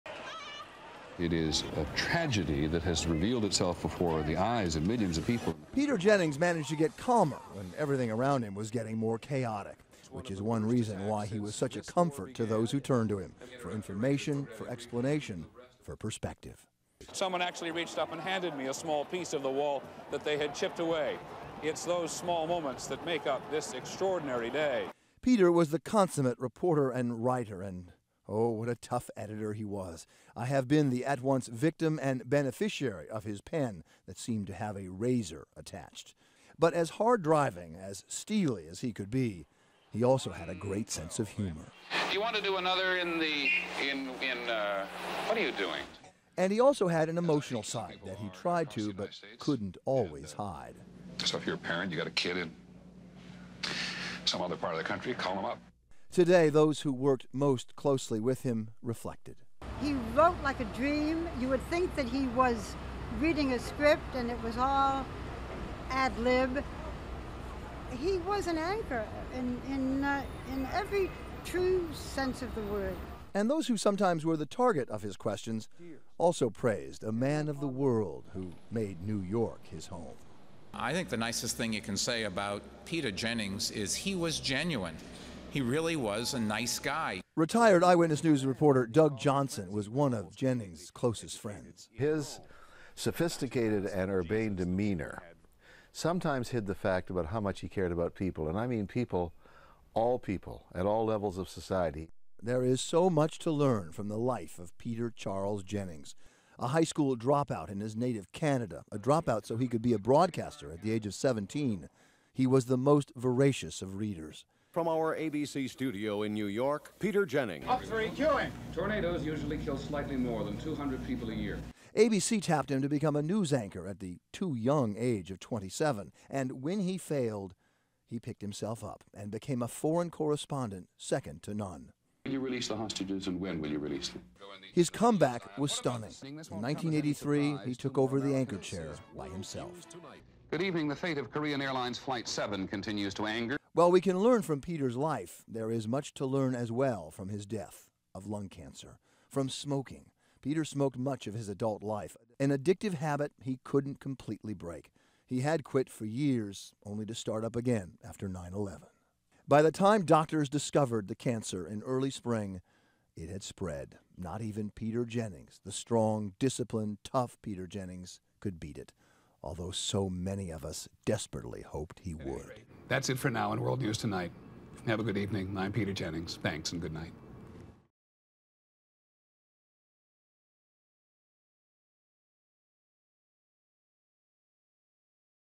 LOOK BACK: Remembering Peter Jennings on the day he died
Bill Ritter looks back at the life of Peter Jennings, who passed away on August 7, 2005 leaving behind a legacy spanning four decades. He died after a battle with lung cancer at the age of 67.

NEW HERE? –